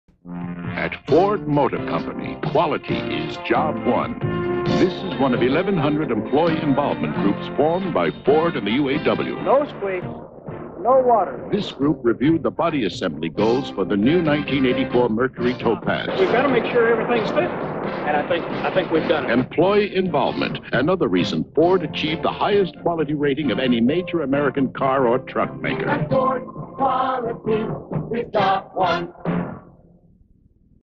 Ford Quality is Job 1 commercial from 1983
Pulled from VHS tape of May 8th 1983 broadcast of KXLY Spokane (ABC)